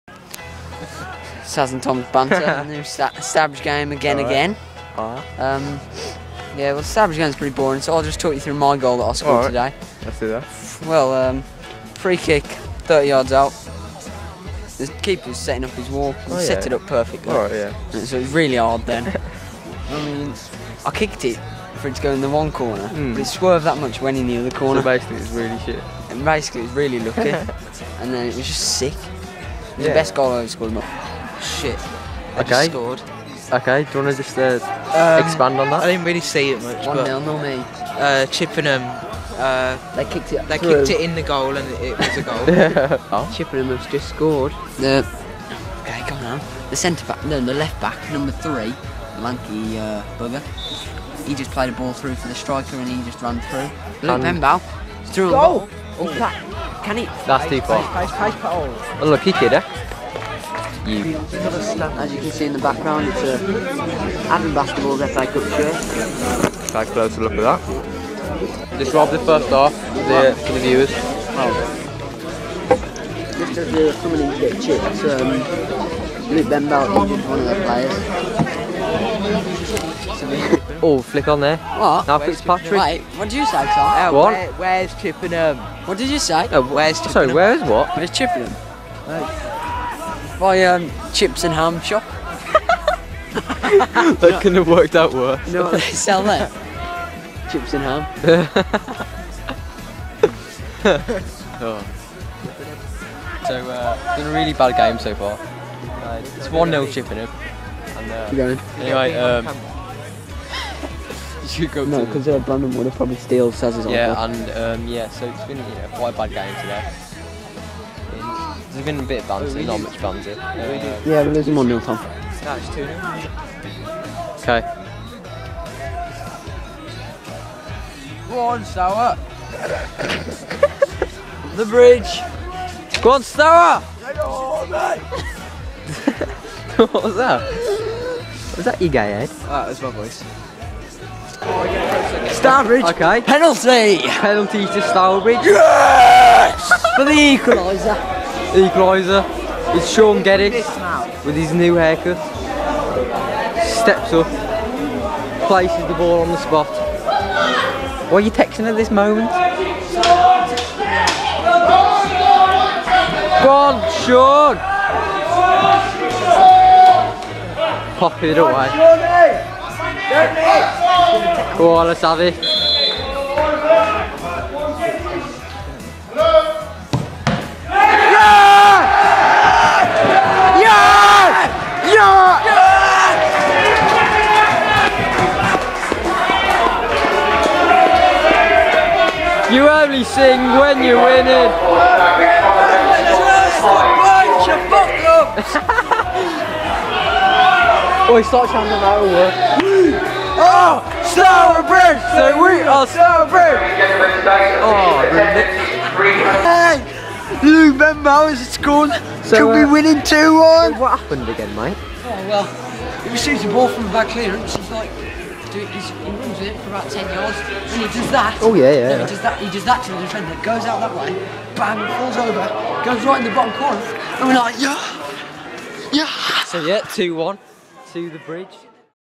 Stourbridge win!
stourbridge came back from 1 - 0 down against chippinam with a penalty from geddes and a great last minute finish from luke benbow.